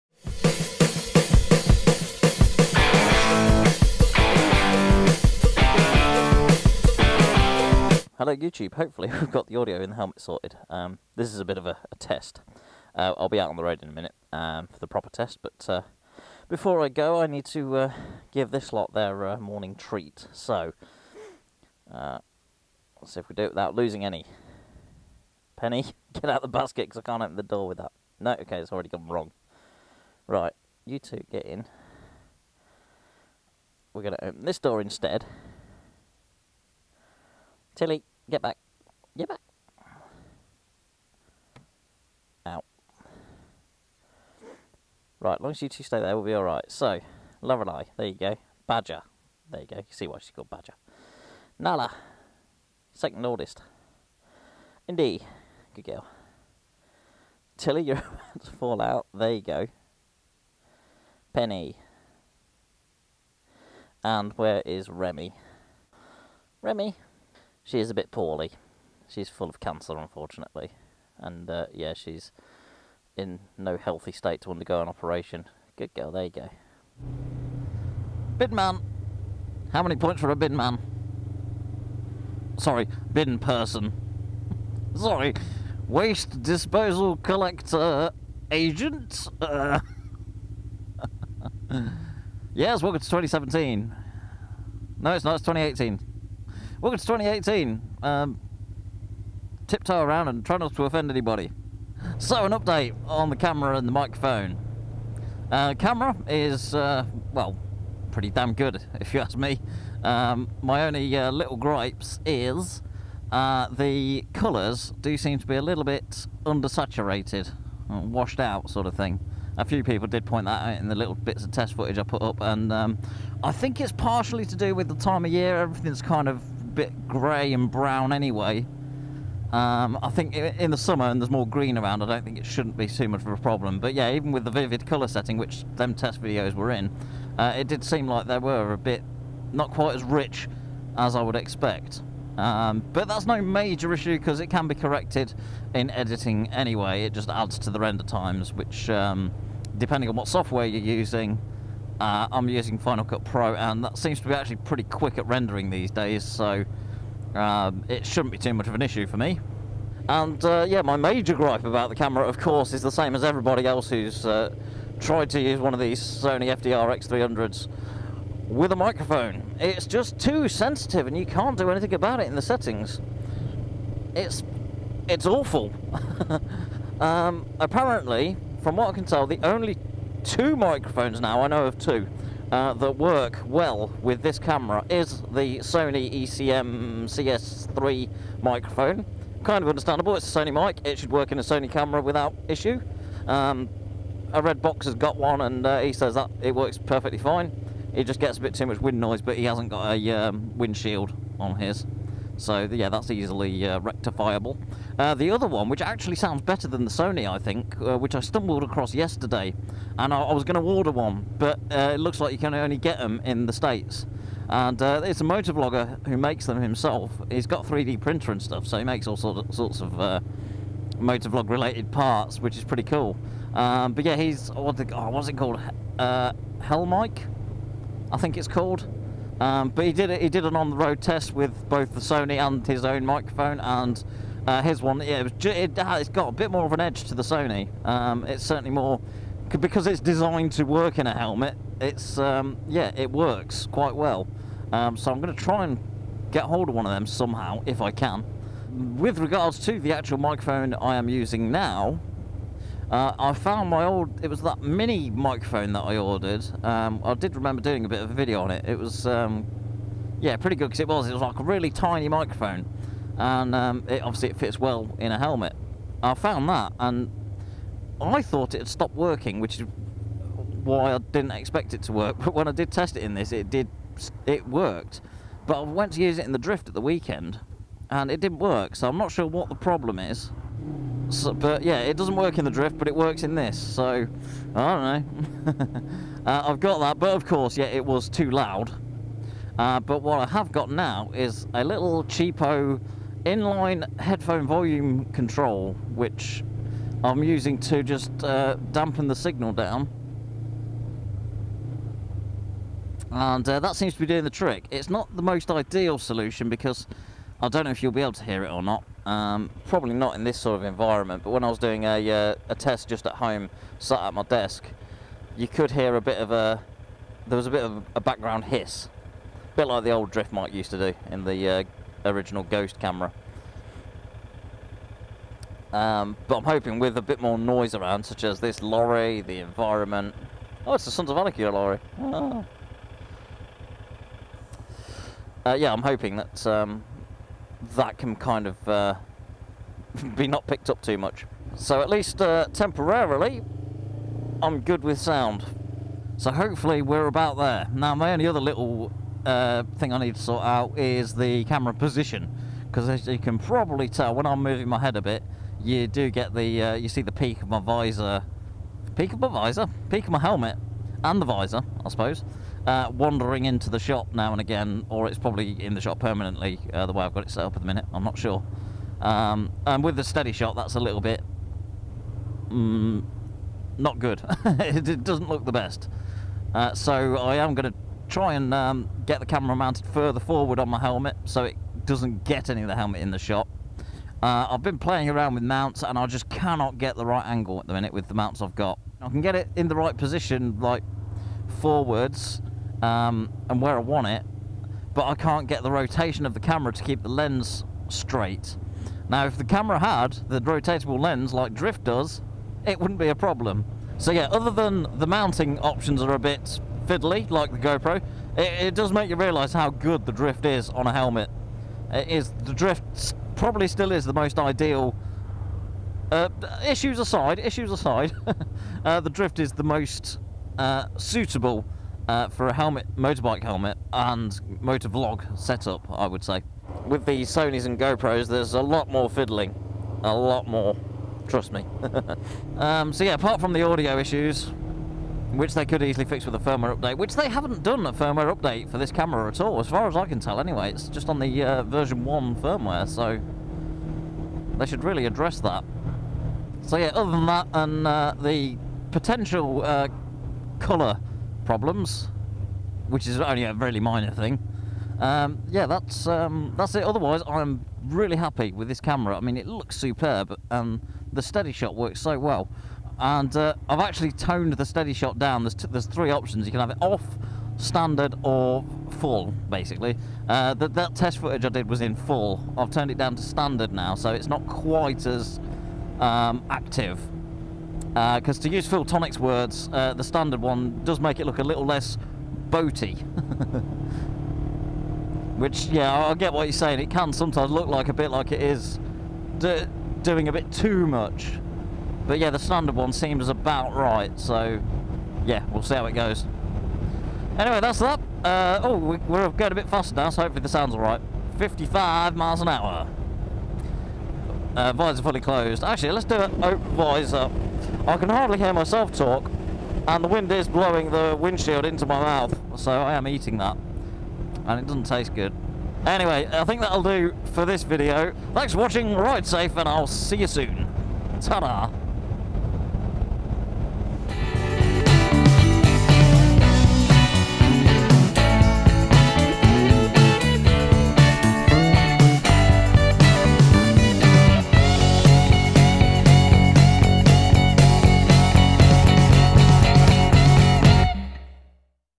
How I'm Getting On With The Sony FDR-X3000 & Its Audio Issues
How I'm Getting On With The Sony FDR-X3000 & It's Audio Issues...
Warning, may contain Rats.
I have at least found a temporary solution to the audio problem in my new camera.  For now, I'm using one of my old microphones that I found, with an inline volume control.  It is not the perfect solution, but certainly makes the footage useable.

Bikes:
2015 Triumph Scrambler
2000 Kawasaki GPZ 500 S

Helmet Camera: Sony FDR-X3000
Secondary Cameras: Drift Ghost S, GoPro Hero 3 White